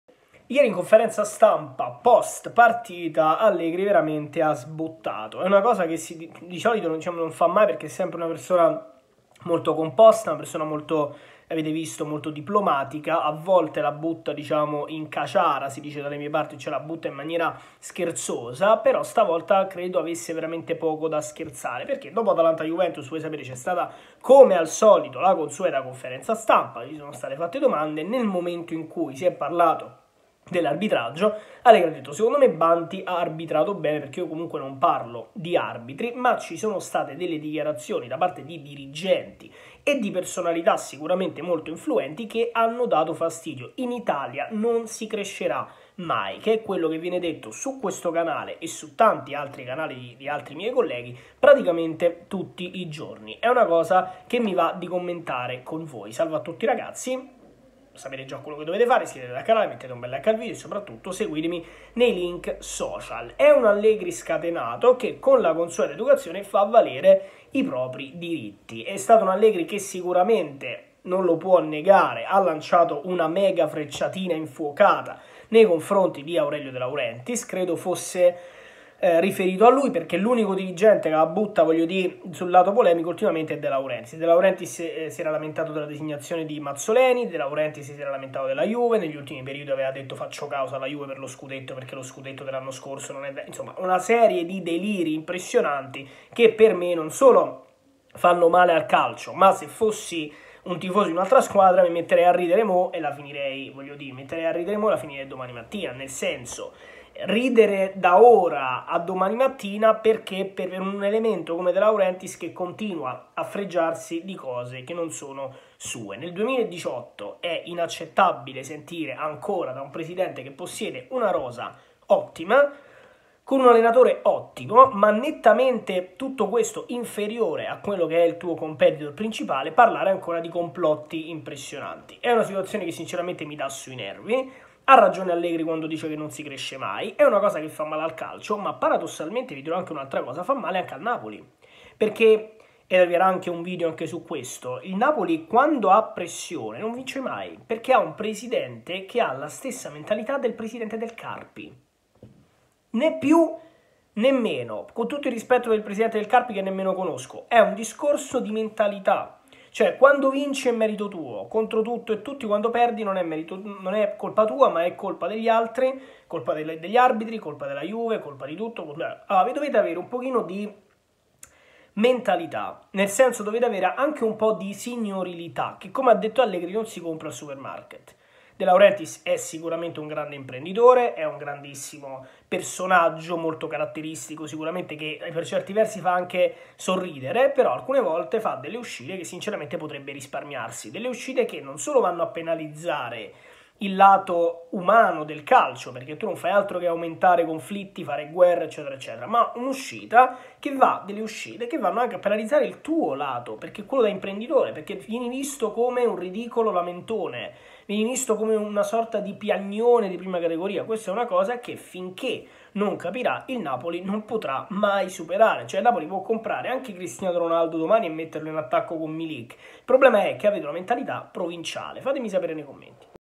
ALLEGRI MULTATO PER AVER DETTO LA VERITÀ
Allegri multato per aver pronunciato dichiarazioni insinuatorie nei confronti della Procura, come mai De Laurentiis, che da anni parla di campionato falsato, viene ignorato?